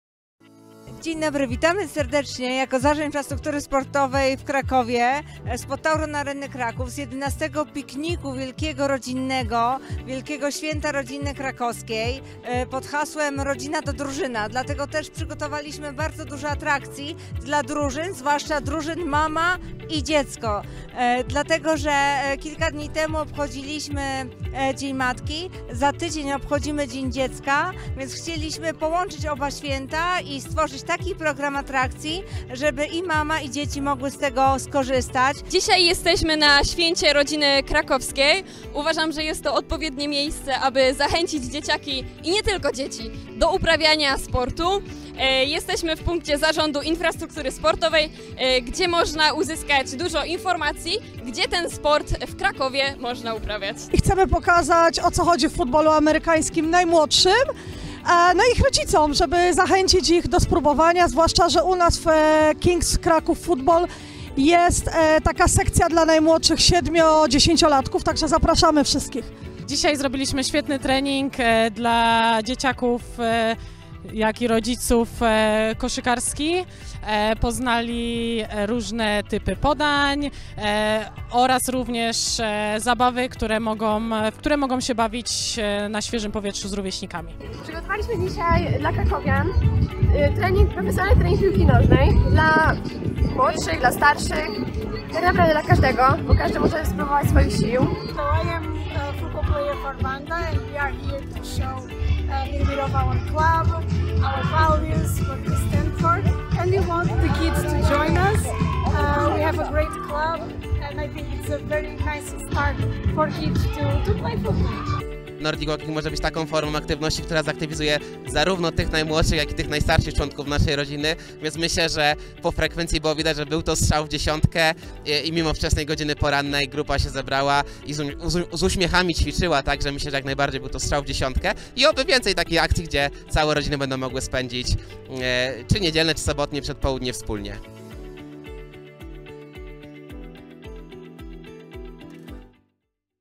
Finał XI edycji Święta Rodziny Krakowskiej
Za nami XI edycja Święta Rodziny Krakowskiej, wydarzenia promującego wartości rodzinne, aktywny tryb życia, możliwości ciekawego spędzenia czasu oraz Kraków, jako miasto przyjazne rodzinie.👨‍👩‍👧‍👦

W niedzielę, 28 maja, podczas wielkiego finału na terenach zielonych przy Tauron Arenie Kraków, była możliwość wzięcia udziału w treningach i zajęciach ruchowych organizowanych przy stoisku Zarządu Infrastruktury Sportowej w Krakowie. 🏃‍♀️ 🏃 🏈 ⚽️

Dziękujemy, że byliście z nami:
Grzegorz Sudoł G&G Sport
Jakub Deląg Nordic Walking
Kraków Football Kings
RadwanSport
KS Wanda Kraków